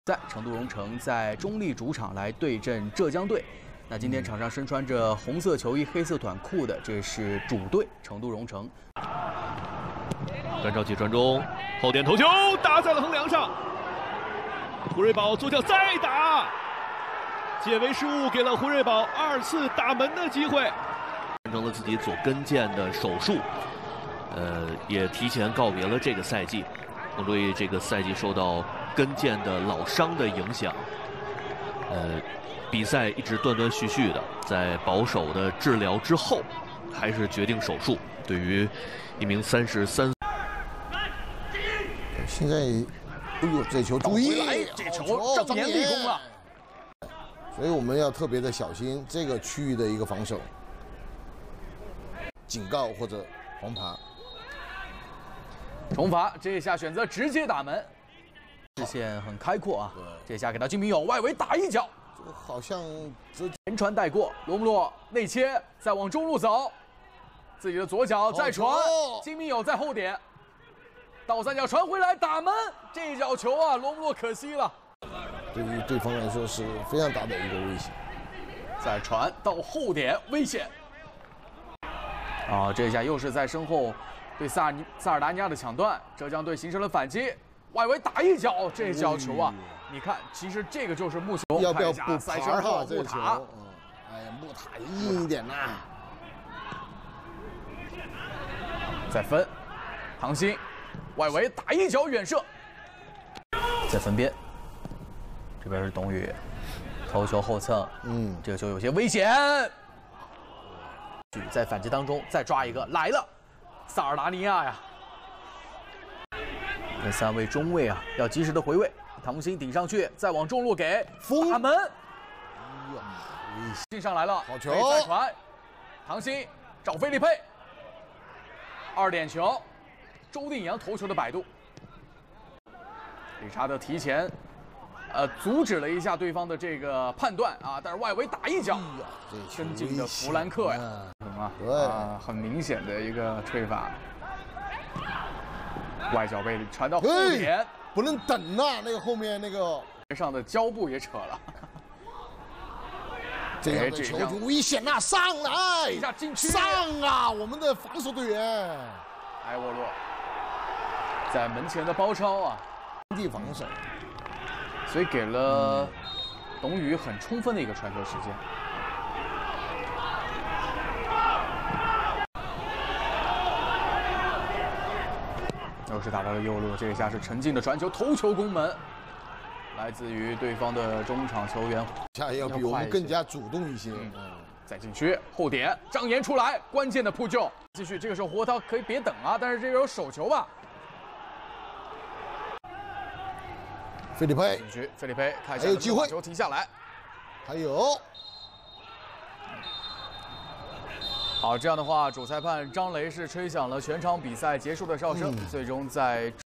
中超集锦：成都蓉城vs浙江队，胡睿宝破僵，埃沃洛杂耍脚后跟垫射
中超第19轮比赛，成都蓉城迎战浙江队。上半场，胡睿宝补射造成董宇自摆乌龙。下半场，董宇助攻埃沃洛脚后跟破门扳平比分。全场比赛结束，成都蓉城1-1浙江。